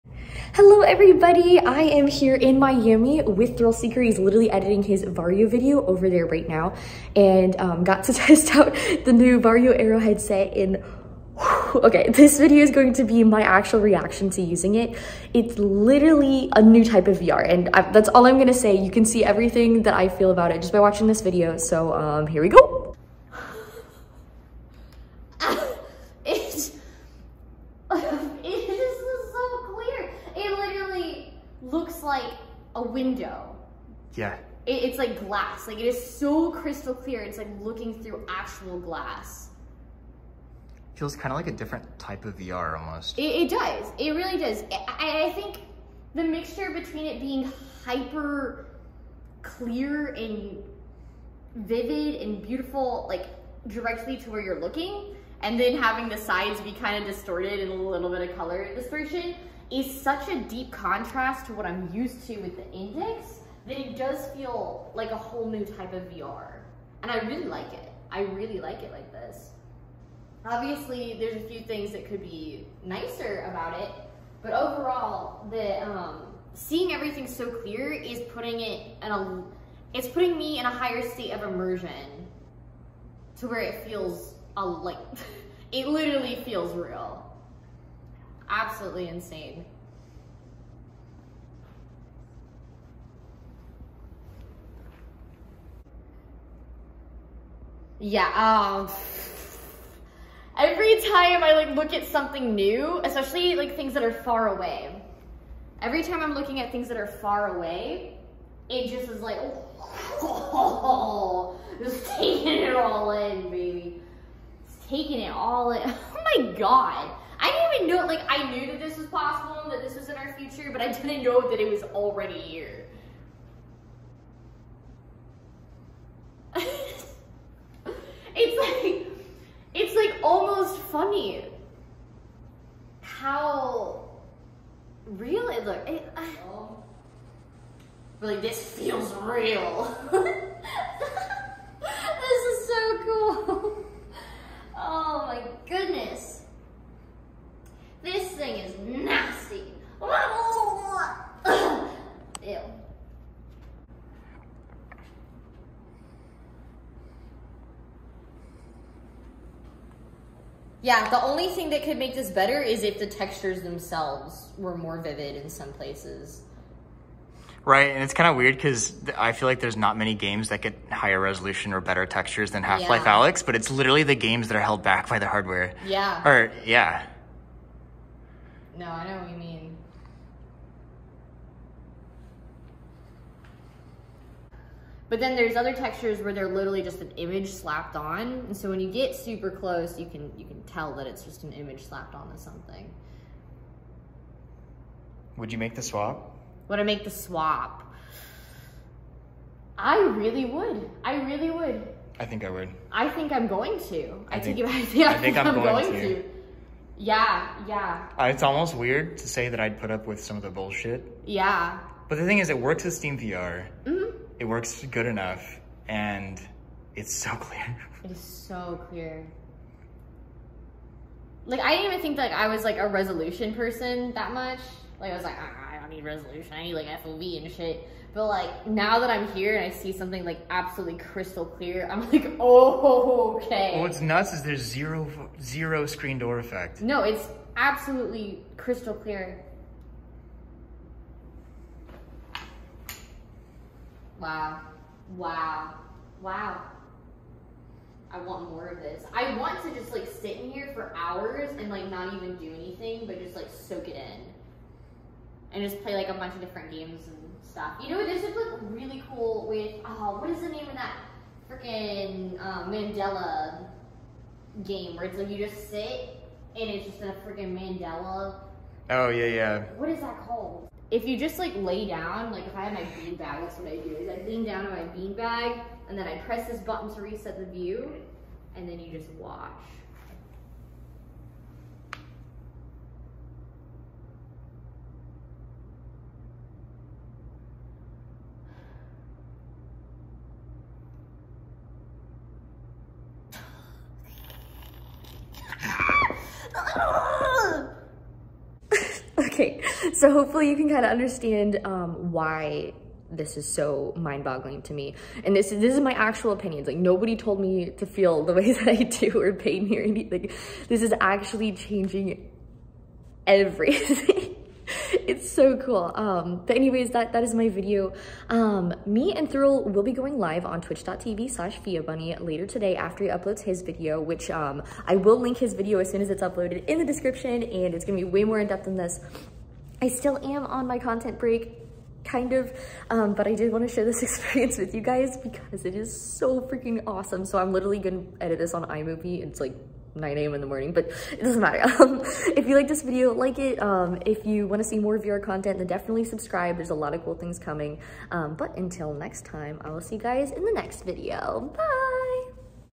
First Reaction Trying the VARJO AERO… (NO SCREEN DOOR?!)
0 screen door, and the most crystal clear VR experience I’ve ever tried… I literally didn’t even know this tech existed already. This is my raw first impression of the Varjo Aero, and it’s not like ANYTHING I’ve ever tried before. Honestly, my entire outlook on where VR is going is kinda flipped upside in the COOLEST way. Please enjoy my initial reaction!

We both will be streaming on my channel later today, so drop a follow and see you there :)